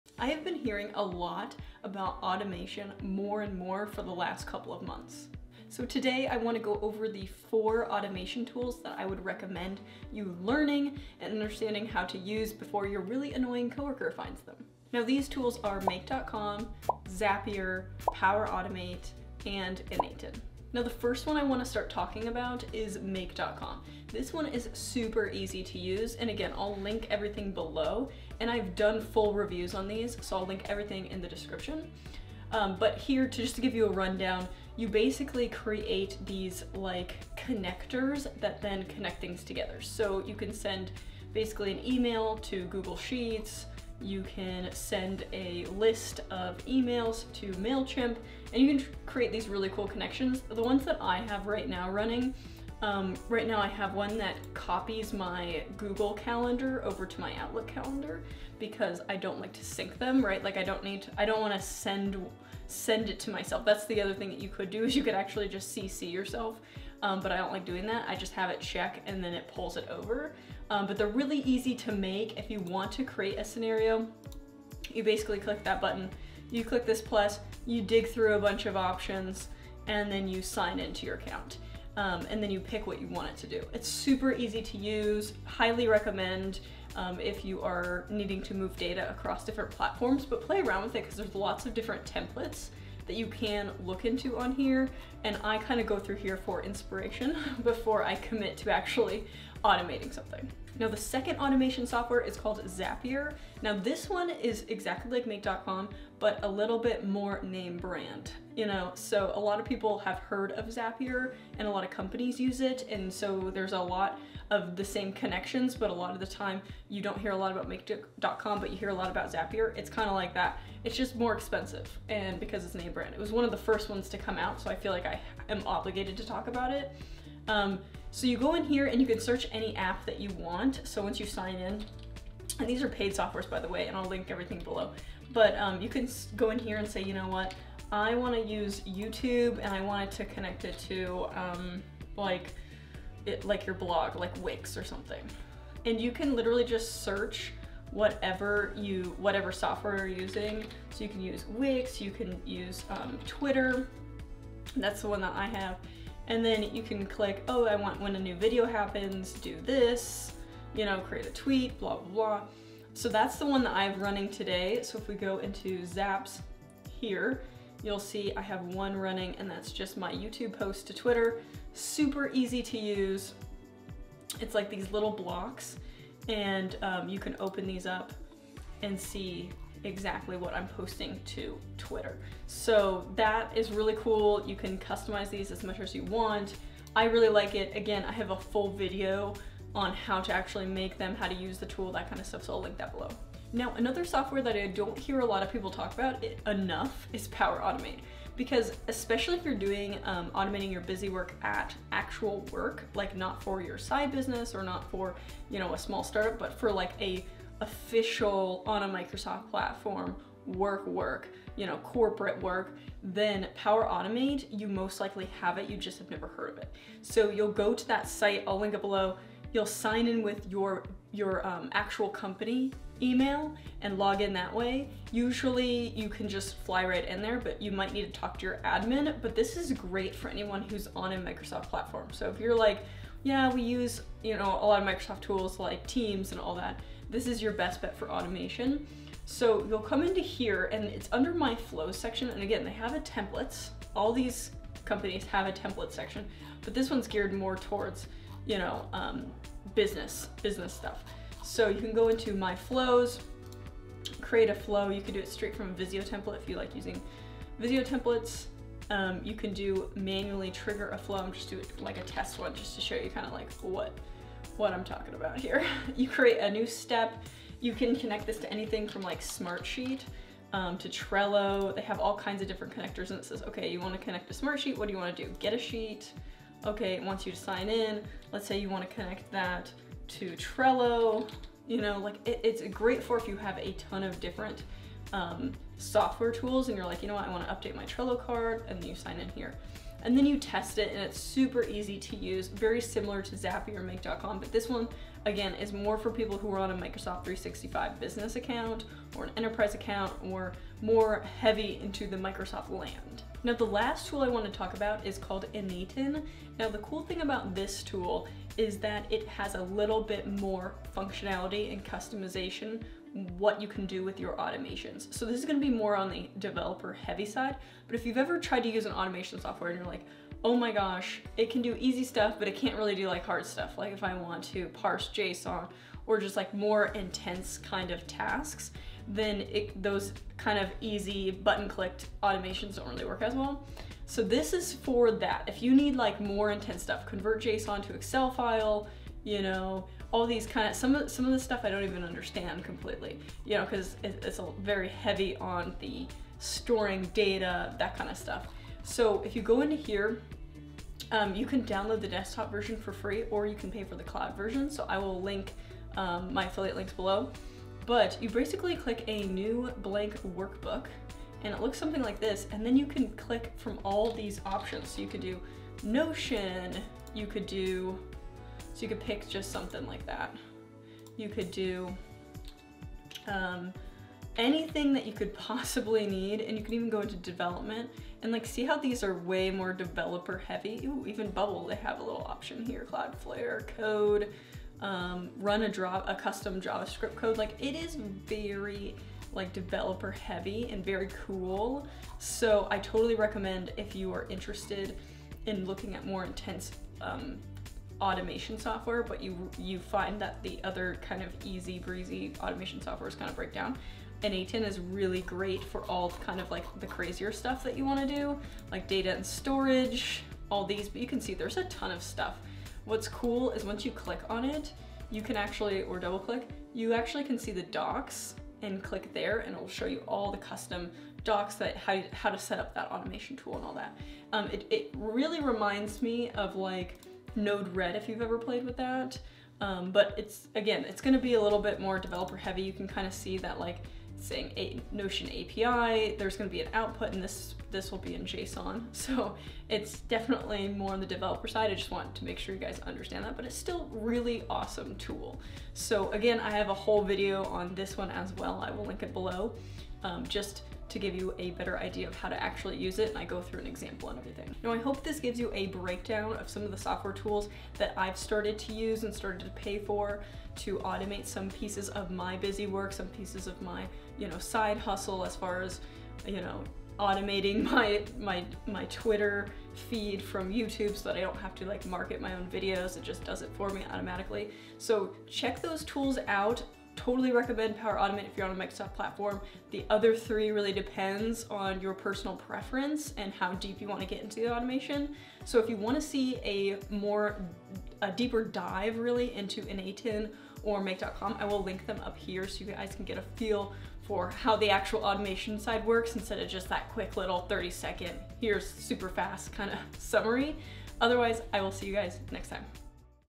💻 4 Life Changing Tools to Automate Your Daily Work!
🎬 VIDEO CONTENT:
00:00 - Intro
00:21 - Make
01:41 - Zapier
05:04 - n8n
07:35 - Competitors